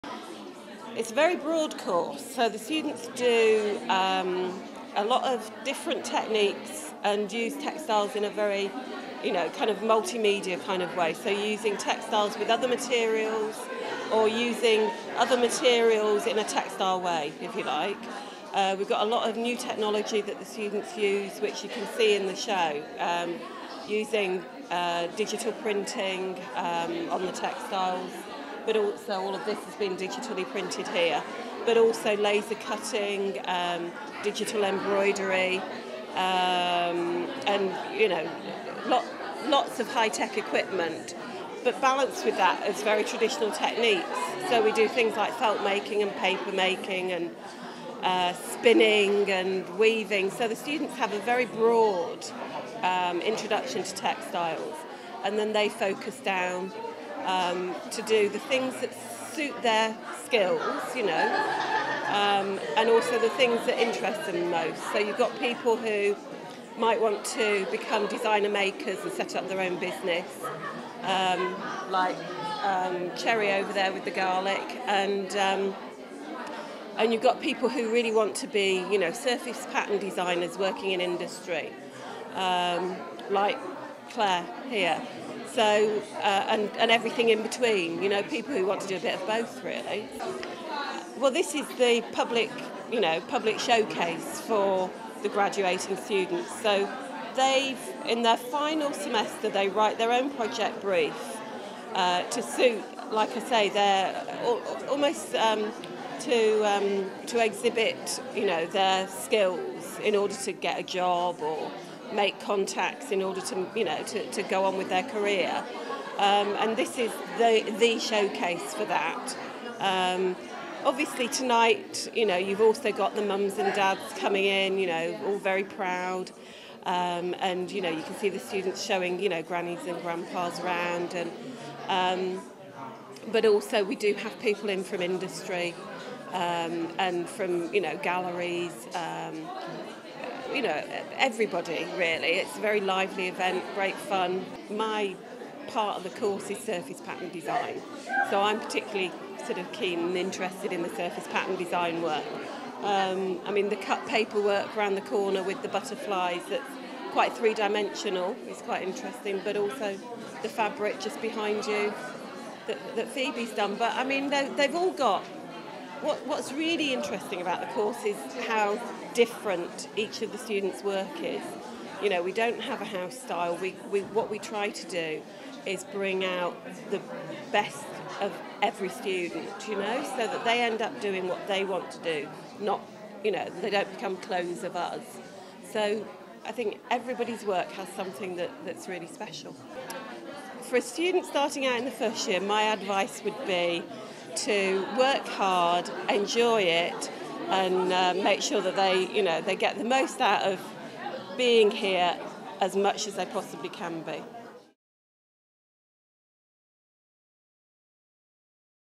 Textile Innovations - Bev Lamey - UCLan
Textile Innovations, Course Leader, Bev Lamey, talks about the course and the work of graduation students at the 2010 UCLan Degree Show.

The top artistic talents of UCLan students were showcased in a two month spectacular.

Fashion, design, photography, music, architecture, fine art, journalism and acting are just some of the course areas being displayed at the UCLan Creative Festival 2010, which took place in July.

The best of photography, interiors and advertising will be display at The 2010 Free Range Art and Design Show in London. The show provides the best platform for graduate art and design students to showcase their work to both public and industry.

The DNA Show (Designers Northern Alliance) provides the best platform for the BA(Hons) Graphic Design students to showcase their work to the public and industry sector while the work of textiles, ceramics and product design undergraduates will be on display at the New Designers show in London.

Glenda Brindle, Head of Creative and Performing Arts, said: "Art, design and performing arts courses at the University rank amongst the best in the UK while many of our students have gained prestigious national and international awards. The Creative Festival is a fantastic opportunity for members of the public to see the excellent, original work these students have produced while potential employers can see the quality of what our soon-to-be graduates have created."